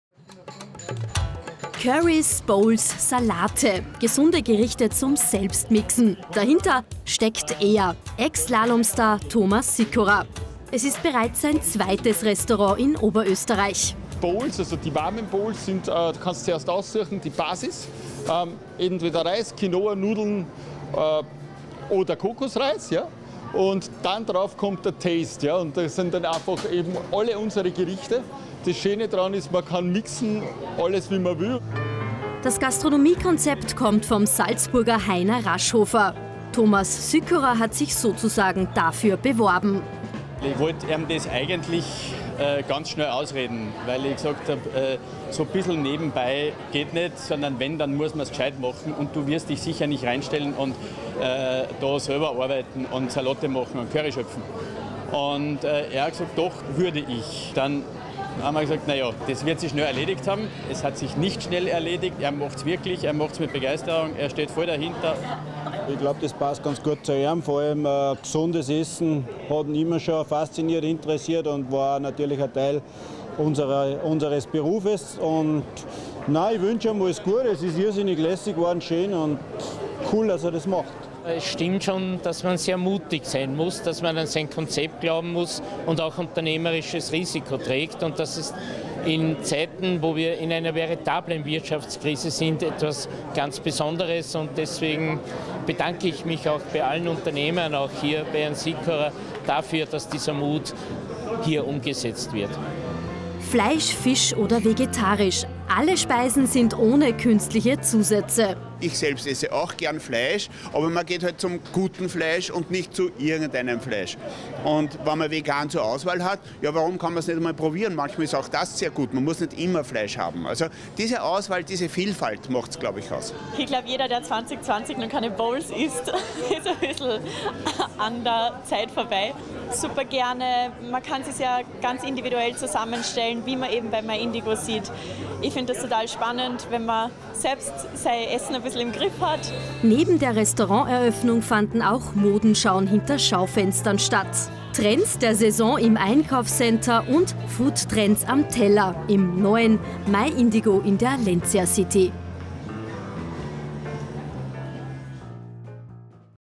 Bowl-Hype - Thomas Sykora eröffnet zweites Lokal
In Krisenzeiten ein Restaurant zu eröffnen. Der „spinnt“ doch sagen die einen. Andere wiederum sagen,  jetzt ist die beste Zeit dafür. Ex-Skirennläufer Thomas Sykora hat genau das gemacht. „My Indigo“ ist sein neues Restaurant. Gesunde Gerichte gibt’s ab jetzt in der Lentia City in Urfahr.